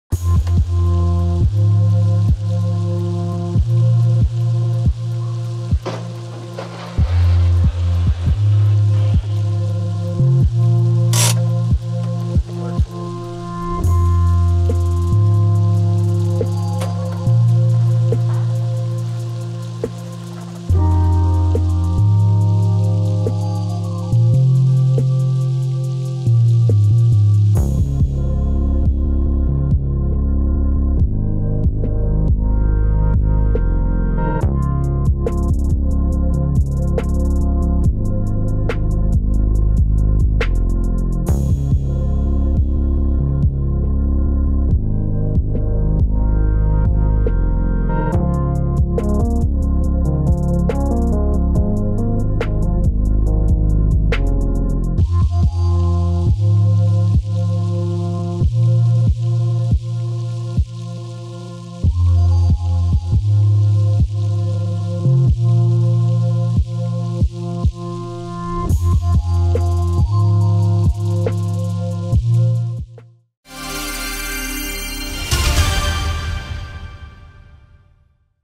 Тройное ДТП с погибшим и пострадавшими произошло в Череповецком районе
Трагедия произошла 20 августа в 19:05 на 2 км автодороги «Череповец-Белозерск». Микроавтобус «Хёндай» занесло на мокрой трассе. Водитель 1966 г.р. не справился с управлением, выехал на «встречку» и столкнулся с «Шевроле». После этого микроавтобус откинуло ещё и на «Ладу-Калина». В результате ДТП погиб пассажир «Шевроле», ещё шесть человек получили различные травмы.